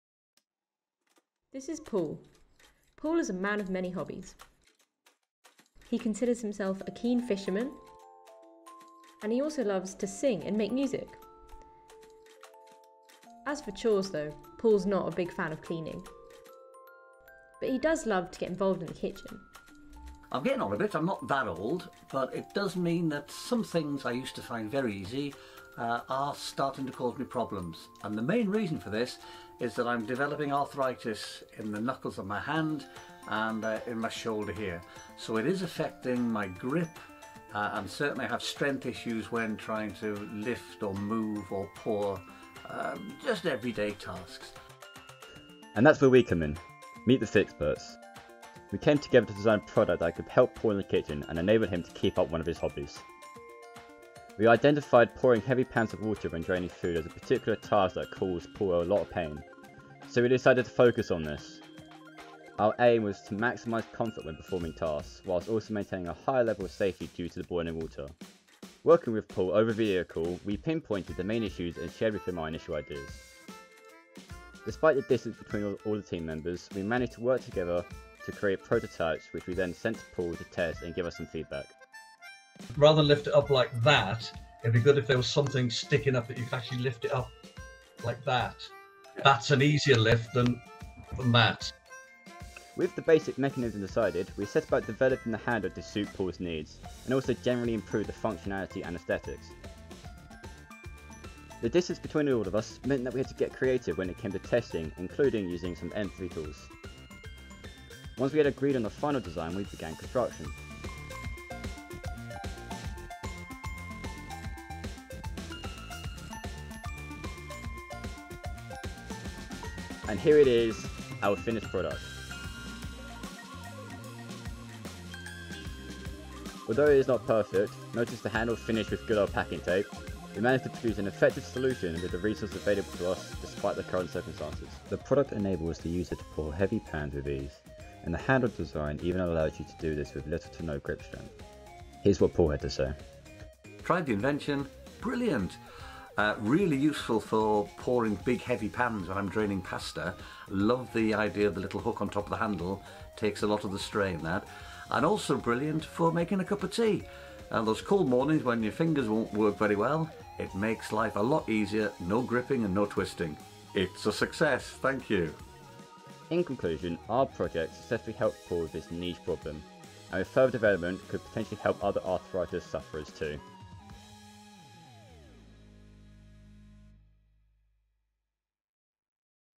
Cooking with Paul
Fix Partner Paul is a man of many hobbies including fishing, singing, making music and cooking. Paul has developed arthritis in the knuckles of his hand and shoulder which affects his grip. Fixperts from Brunel University London came together virtually during the pandemic with Paul to create a utensil which would help to make cooking enjoyable and safe.

Fixperts: Marwan Ali, Annabel Connah, Scott Heathfield, Nuoya Ma, Eleanor Mawdsley and Alfs Pupols
Fix Partner: Paul
Master Fixperts: Mike Kann
Education Institute: Brunel University London
Country: England
February 2021